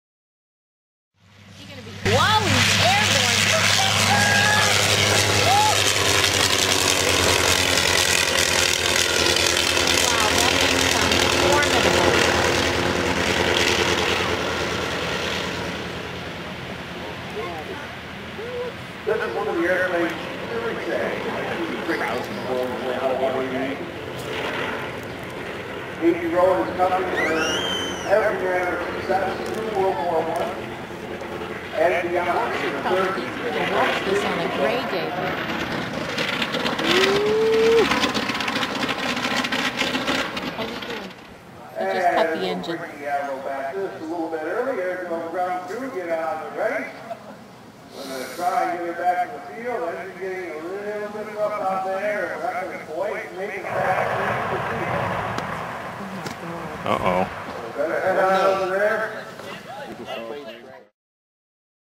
Rhinebeck Aerodrome AVRO 504K crash
WWI-era AVRO 504K biplane crashes during air show at Rhinebeck Aerodrome, Rhinebeck, NY, USA, October 17, 2009.  Just after takeoff, the engine began to run rough, then quit.  The pilot was unable to reach the runway, and crashed into trees just beyond.  Aerodrome staff reported the pilot was OK.  The aircraft had only been restored a couple of months earlier.